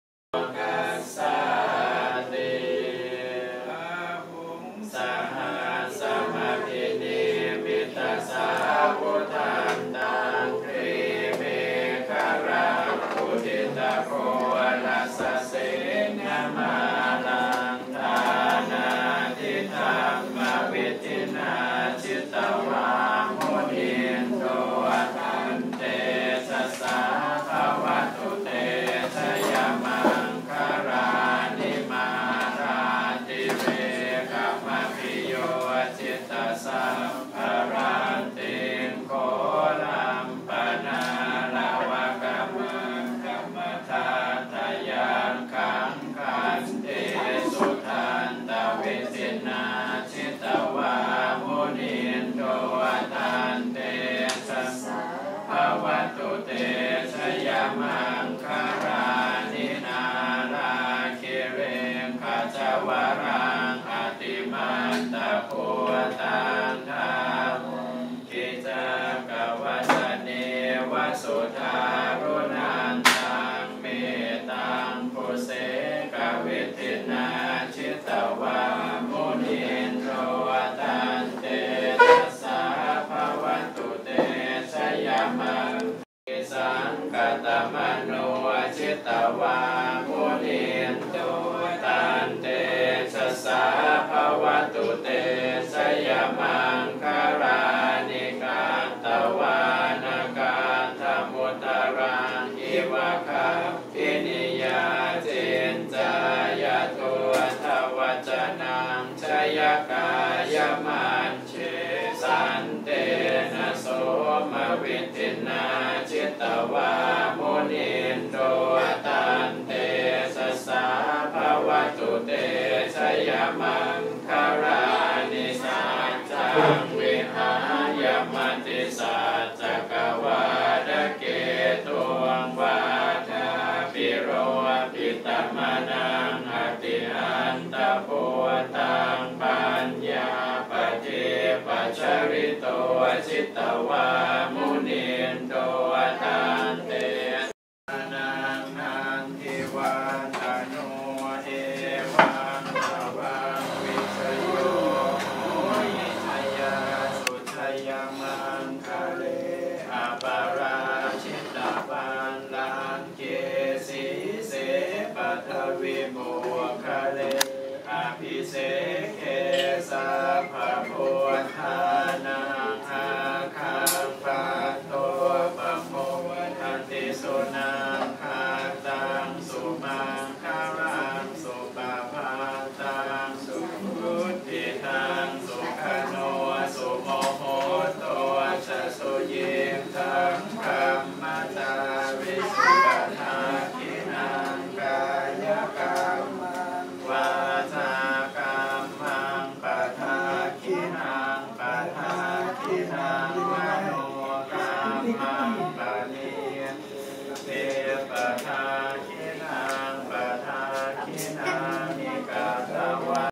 1 year anniversary of Min Thai Takeaway
Min Thai Takeaway celebrates its first anniversary with a visit from three buddhist monks.